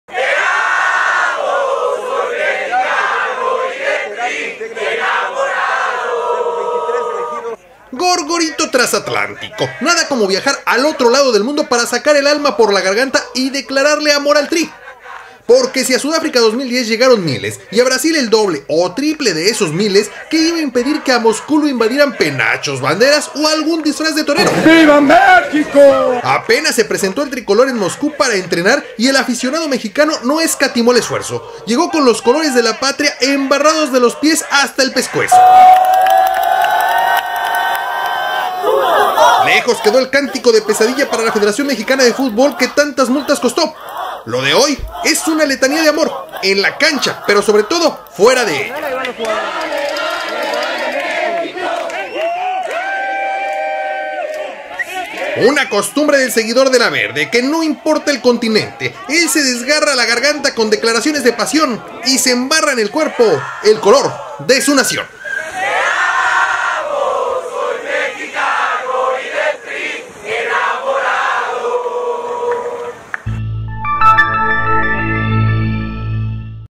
Cantos de amor de la afición al Tri en Moscú
Moscú, Rusia. Decenas de aficionados se dieron cita en el entrenamiento de la tarde del martes en Moscú, que la Selección Mexicana organizó a puertas abiertas y de dejó apapachar por el entusiasmo de sus seguidores.